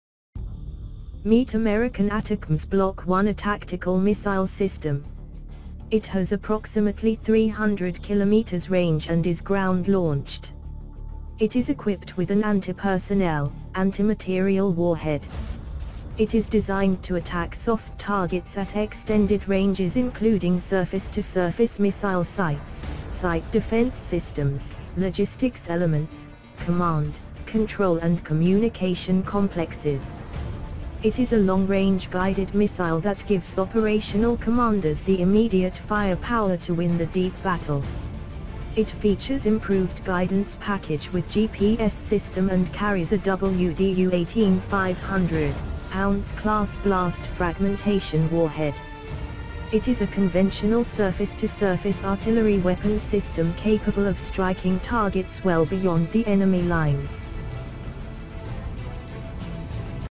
The ATACMS Block-1A Tactical Missile System of United States.
ATACMS Block-1A Tactical Missile System of United States.

It approximately 300km range. Ground-Launched. Equipped with an Anti-Personnel / Anti-Material (APAM) warhead. Designed to attack soft targets at extended ranges including SSM sites, Site defense systems, logistics elements, command, control and communications complexes. It is a long-range guided missile that gives operational commanders the immediate firepower to win the deep battle. It features improved guidance package with GPS system and carries a WDU18 500lbs Class Blast Fragmentation Warhead. It is a conventional surface-to-surface artillery weapon system capable of striking targets well beyond the range of existing Army cannons, rockets and other missiles. ATACMS missiles are fired from the MLRS M270 and M270A1 weapons platform.

The Block-1A weapon delivers a monolithic high-explosive warhead. It has vertical impact capability and collateral damage is much less than 100m from the point of impact.